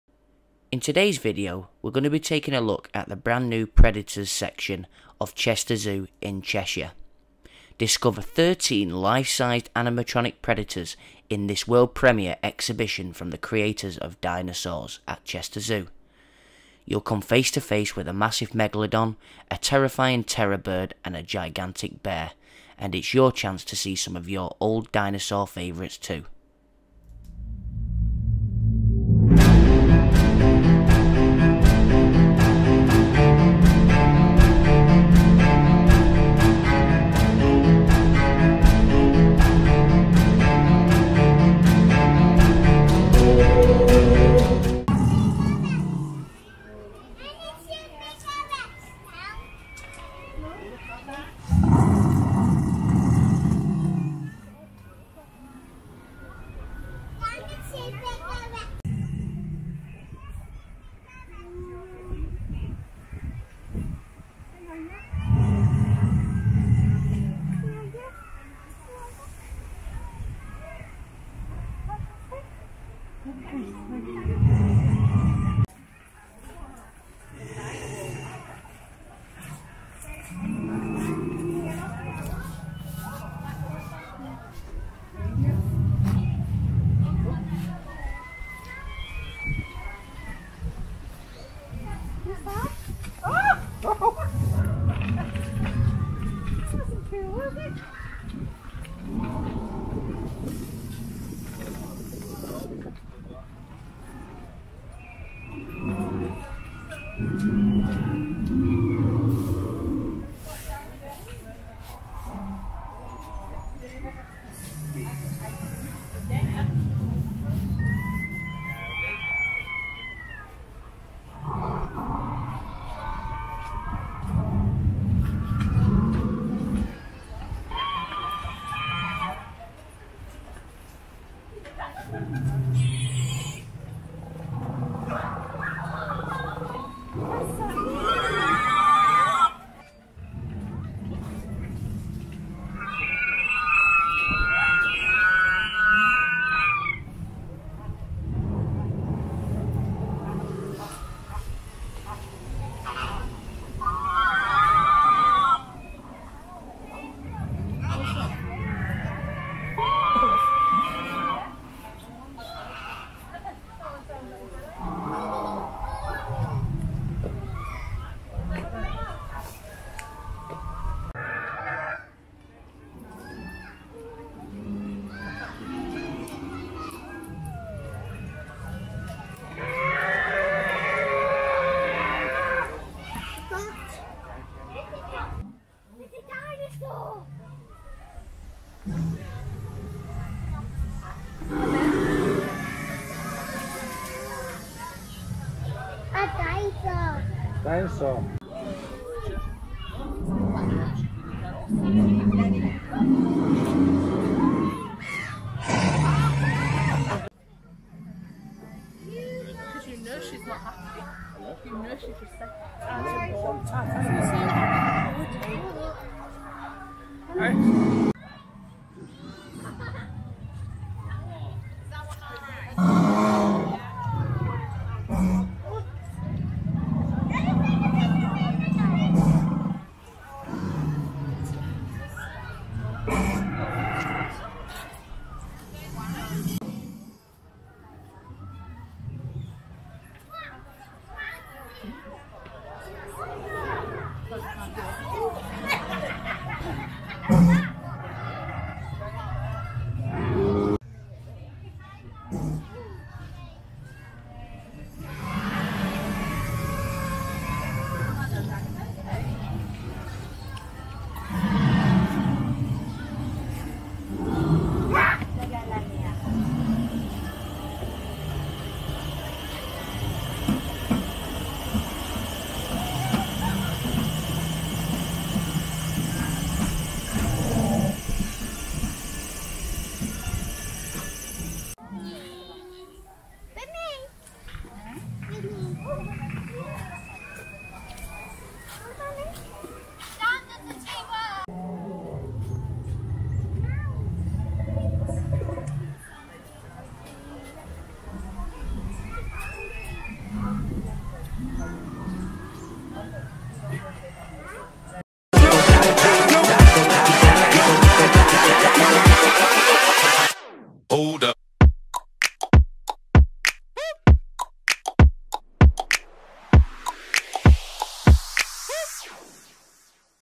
Predators at Chester Zoo Walkthrough
Thirteen enormous animatronic predators have arrived at Chester Zoo, ahead of the opening of a world first exhibition.
The giant robotic hunters were delivered to the zoo on trucks after being shipped across the Atlantic from the USA.

Featuring the infamous dire wolf – one of the most feared land predators of all time – a giant 12-foot-tall bear and a sabretooth cat, the long-lost mega beasts will be on display for the first time when the zoo’s Predators exhibition made it's world premiere on Saturday, May 26.